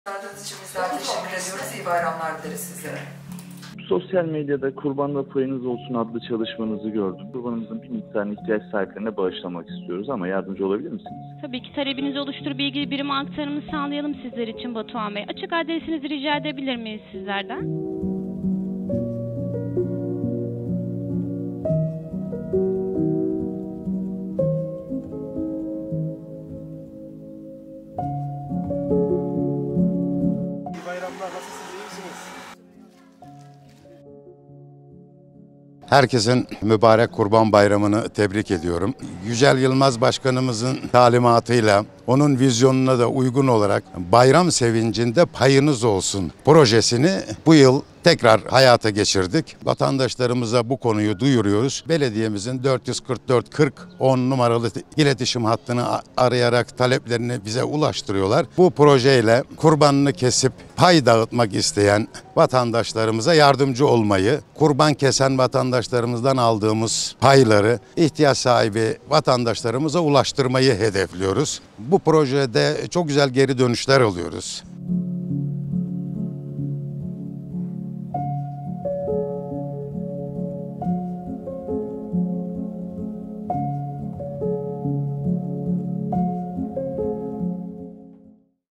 Büyükşehir, ihtiyaç sahiplerine Kurban paylarını ulaştırıyor
Bayram Sevincinde Payınız Olsun

Balıkesir Büyükşehir Belediyesi her Kurban Bayramı’nda olduğu gibi bu bayramda da payını dağıtmak isteyen vatandaşlarla, pay almak isteyen vatandaşlar arasında köprü olan “Bayram Sevincinde Payınız Olsun” projesini hayata geçirdi. Kurban Bayramı’nda her eve et girmesi için ekipler, bayram boyunca aralıksız çalışmaya devam edecek.

Kurban Bayramı’nda payını dağıtmak isteyen vatandaşlarla, ihtiyaç sahibi vatandaşlar arasında köprü olan Balıkesir Büyükşehir Belediyesi “Bayram Sevincinde Payınız Olsun” projesini her yıl olduğu gibi bu yıl da devam ettiriyor. Balıkesir Büyükşehir Belediye Başkanı Yücel Yılmaz’ın, Karesi Belediye Başkanlığı döneminden bu yana sürdürdüğü proje ile Kurban Bayramı’nda her eve et girmesi amaçlanıyor. Kurban Bayramı öncesinde tespit edilen ihtiyaç sahibi vatandaşlar ile bayram boyunca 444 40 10 numaralı Balıkesir Büyükşehir Belediyesi Çağrı Merkezini arayarak pay almak istediğini belirten vatandaşların ev adreslerine, pay vermek isteyen vatandaşlardan toplanan etler hijyenik bir şekilde paketlenerek ulaştırılıyor.
EKİP, BAYRAM BOYUNCA 24 SAAT ÇALIŞACAK
“Bayram Sevincinde Payınız olsun” projesiyle ilgili açıklamalarda bulunan Balıkesir Büyükşehir Belediyesi Sağlık ve Sosyal Hizmetler Dairesi Başkanı Ahmet Meydan “Her Kurban Bayramı’nda geleneklerimize uygun olarak bu güzel projemizi devam ettiriyoruz. Kurbanını kesip, pay dağıtmak isteyen vatandaşlarımızdan aldığımız payları, ihtiyaç sahibi vatandaşlarımıza ulaştırıyoruz. Bu proje için çok güzel geri dönüşler alıyoruz. Kurban Bayramı’ndan önce ihtiyaç sahibi vatandaşlarımızı tespit ettik. Ancak yine de pay vermek isteyenler olduğu gibi pay almak isteyen vatandaşlarımız da belediyemizin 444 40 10 numaralı çağrı merkezini arayarak taleplerini iletebilirler. Bununla ilgili bayram öncesinde 24 saat çalışacak ekibimizi kurduk. Bayramın ilk günüden itibaren payları toplayıp, ihtiyaç sahibi vatandaşlarımıza ulaştırmaya başladık.” diye konuştu.